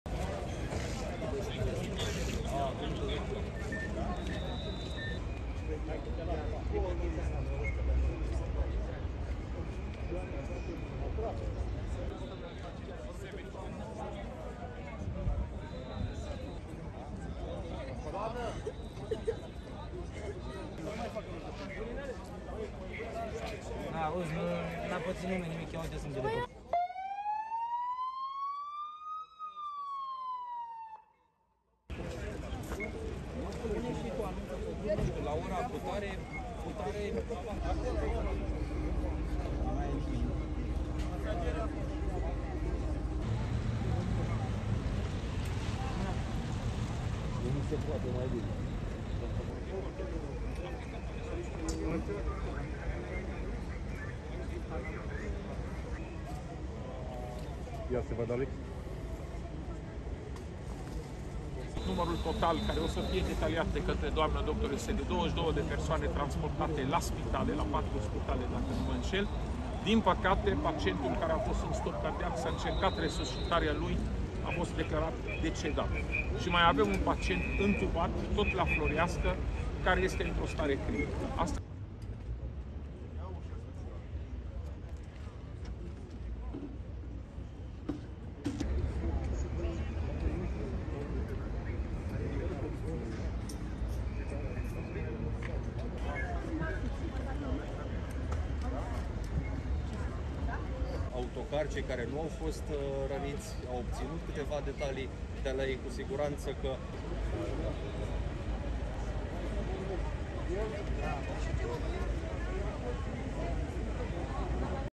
Bus crash in Romania leaves 1 dead, more than 20 injured
RESTRICTION SUMMARY:

ASSOCIATED PRESS
Bucharest, Romania - 23 December 2022
1. Various of damaged bus
2. Various of emergency teams at scene
3. Various of damaged bus
4. Various of emergency teams
5. SOUNDBITE (Romanian) Raed Arafat, Romania's Department for Emergency Situations:
"At this time ,the total number, which will be detailed later, is 22 people transported to four hospitals. Unfortunately, the patient that was in cardiac arrest was declared dead after multiple attempts of resuscitation. There is another patient that is intubated and is in a critical condition."
6. Various of police and emergency teams at site
STORYLINE:
A bus carrying 47 Greek passengers crashed into a height restriction barrier in Romania’s capital on Friday leaving one dead, and injuring more than 20, authorities said.

The incident occurred in central Bucharest at around 1:30 p.m. local time, authorities said.

Romania’s health ministry said in a statement that some of the passengers were trapped inside the bus and that 10 ambulance crews were sent to the scene to provide emergency medical assistance.

Associated Press video showed the roof of the bus crushed underneath the height barrier toward the passenger seats.

All of the passengers, including the driver, are Greek citizens, authorities said.

"The emergency response has ended, Arafat said, adding that the driver remained to provide statements to police.

AP video shot by Vadim Ghirda